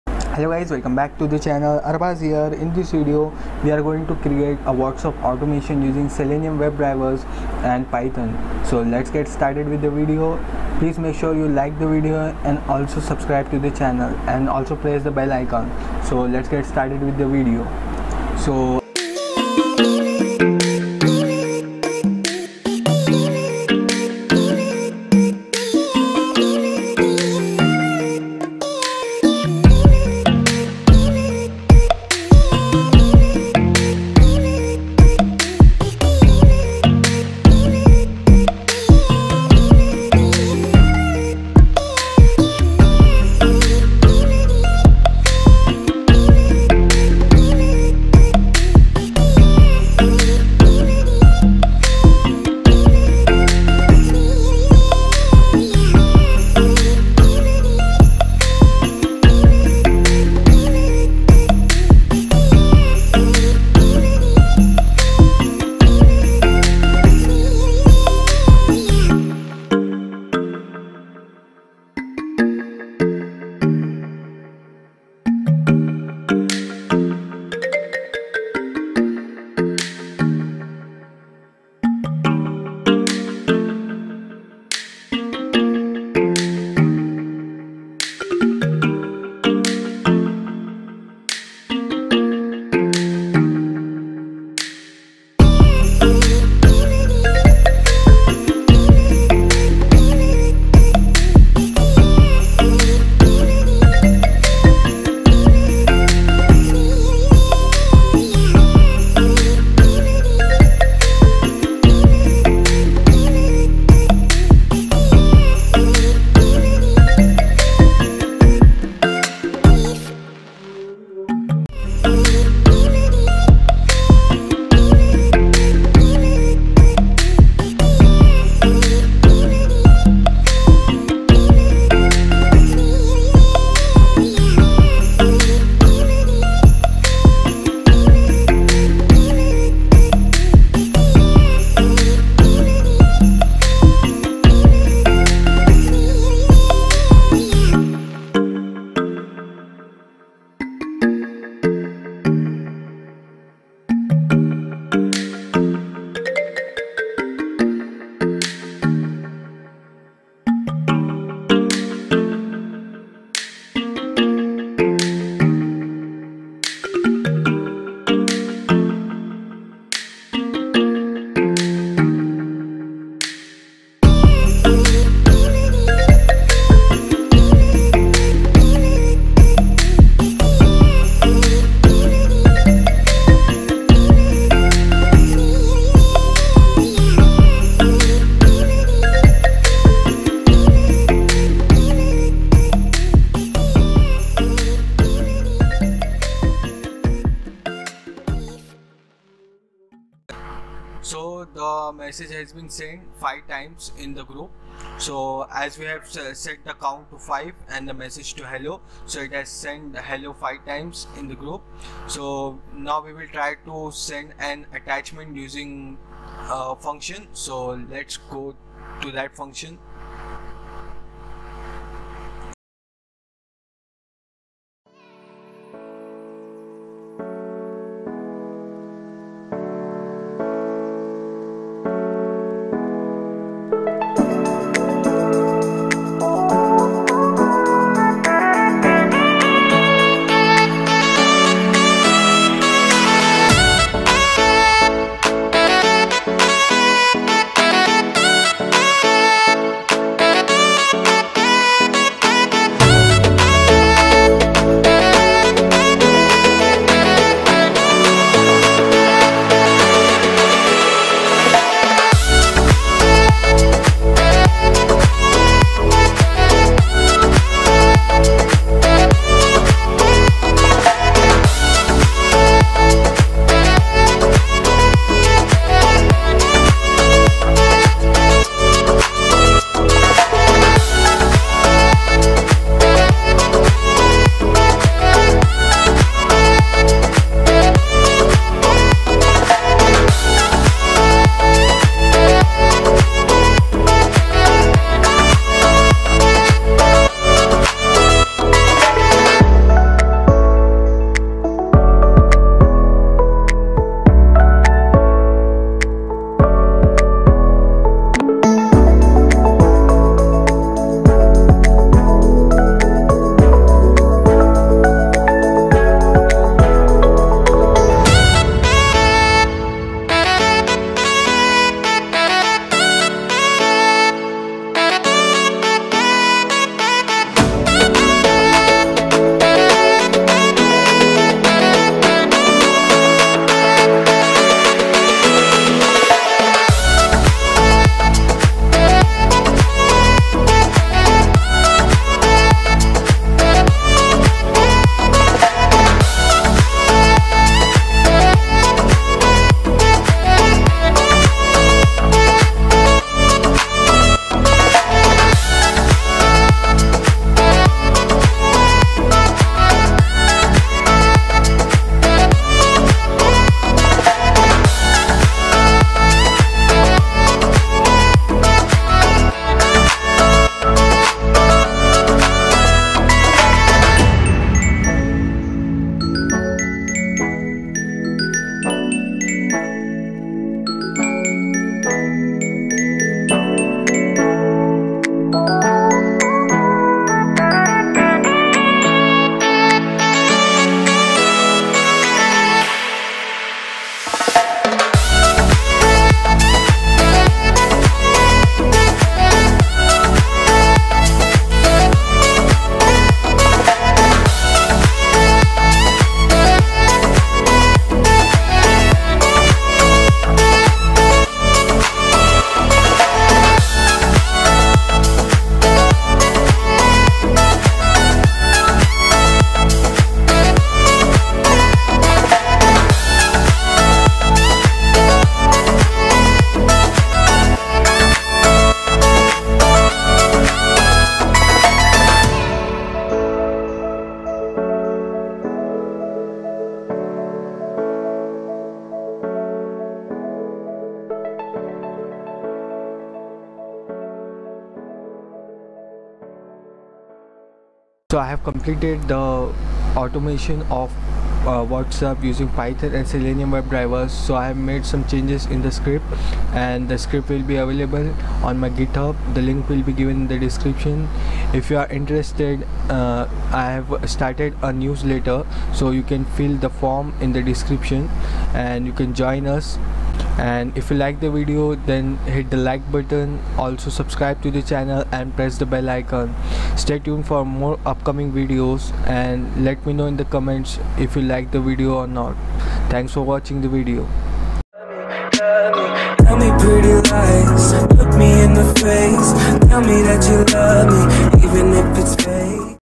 One Day Builds: Whatsapp Automation
In this video tutorial, I have shown how to automate what's app using python & selenium